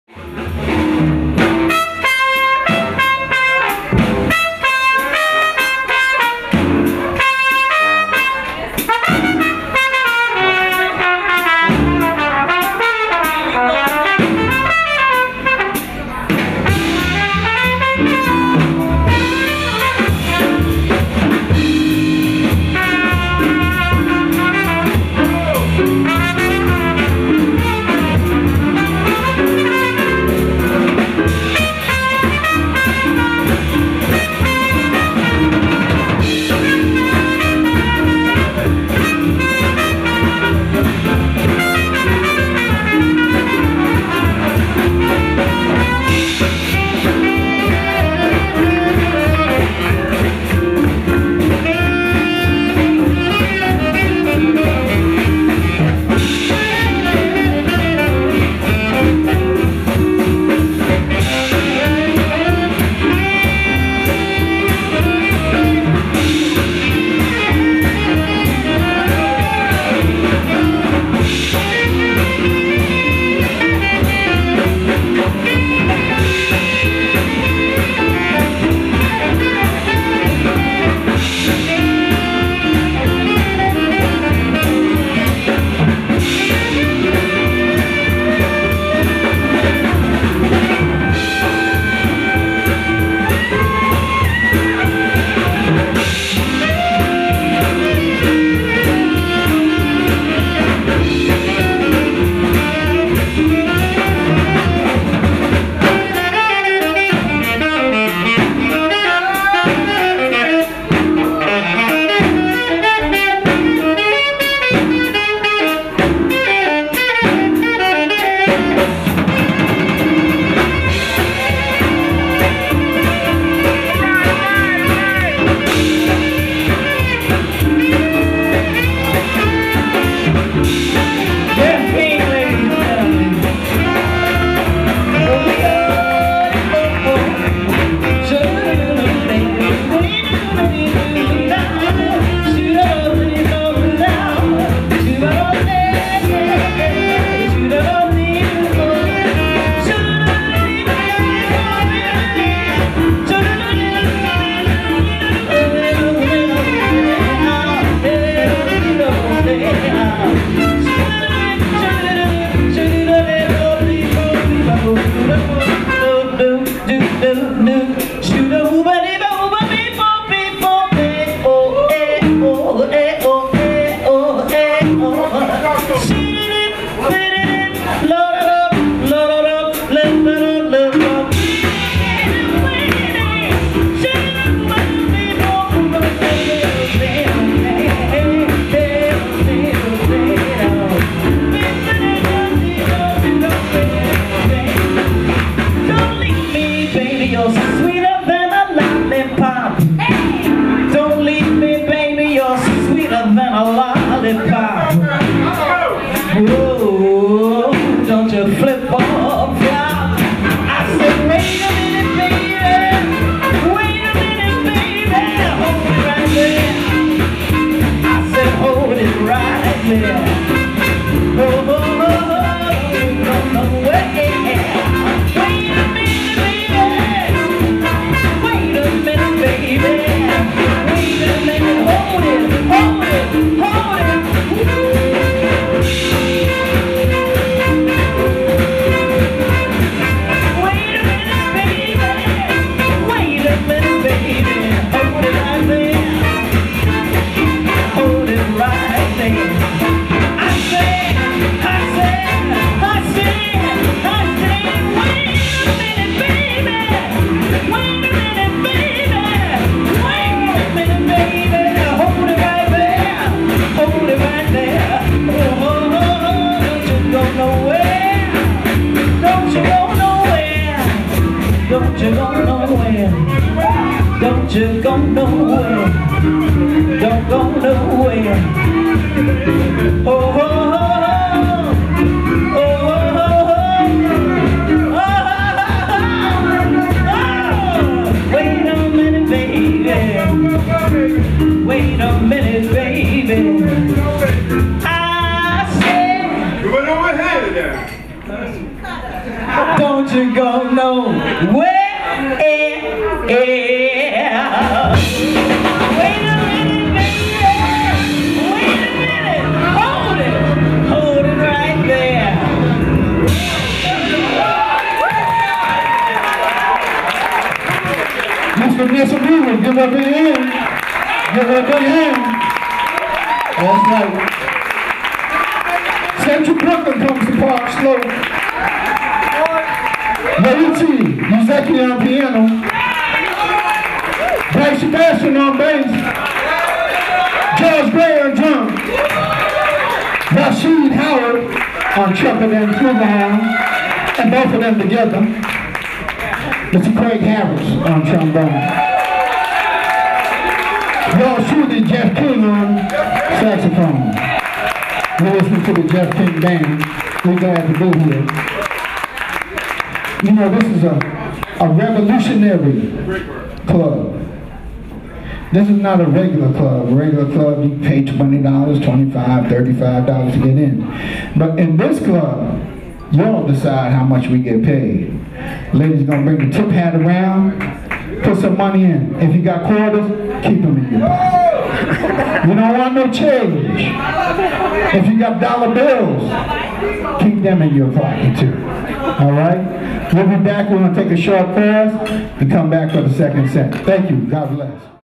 G.B.T.V. CultureShare ARCHIVES 2011: JEFF KING BAND #13 (HD)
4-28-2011 - @ THE TEA CUP, BROOKLYN NY.
JEFF KING... Saxophone
CRAIG HARRIS ...Trombone
RASHEED HOWARD ...Trumpet & Flugel Horn
YOICHI UZEKI ... Piano
BRYCE SEBASTIEN ... Bass
GEORGE GRAY ... Drums
VANESSA RUBIN .. Vocal